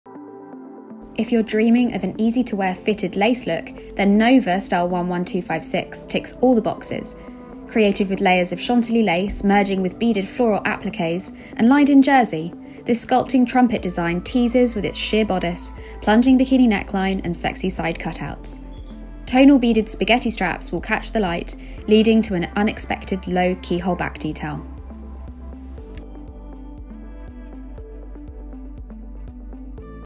11256 NOVA | Adore by Justin Alexander SS23
If you're dreaming of an easy to wear fitted lace look, this wedding dress checks all the boxes. The gown is created with layers of delicate Chantilly lace merging with beaded floral appliqués. Lined in jersey, this sculpting trumpet silhouette teases with its sheer bodice, plunging bikini neckline and sexy side cutouts. Tonal beaded spaghetti straps sparkle as they catch the light, leading to an unexpected low keyhole back detail.